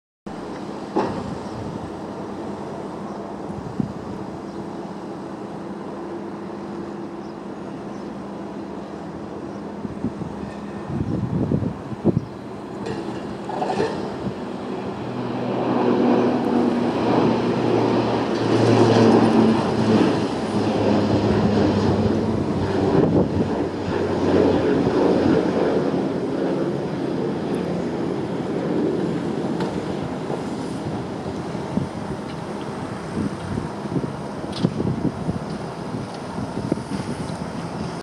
Flybe Flight 325 (BHX to Aberdeen)
Flybe Flight 325 from Birmingham Airport to Aberdeen. The aircraft is an Bombardier Dash 8 Q400 (reg: G-FLBA) is 6.2 years old and has been operating for Flybe since 2009.